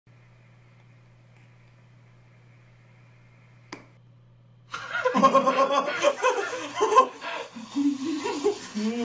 3 guys eat noodles and watch a funny video
epic